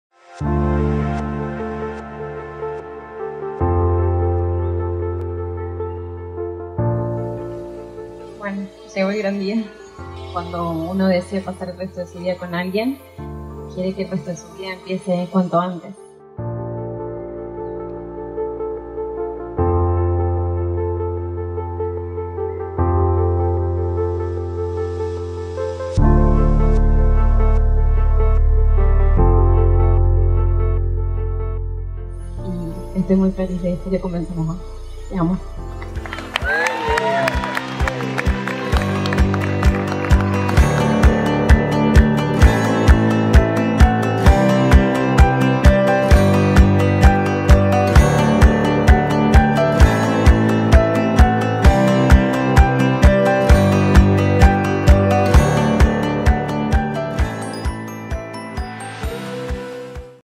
Highlights Boda Lisette y Enzo - La Angelina, Cordoba
Highlights Boda Lisette y Enzo - by CubaLibre.com.ar
Ceremonia y Fiesta: La Angelina - Estancia Contemporanea
Video y Edicion: FerBrown - Cuba Libre
Drone: Gaston Filiberti
Postproduccion: Anabella Cupertino y Fernando Brown - Cuba Libre
Foto de portada: Anabella Cupertino - Cuba Libre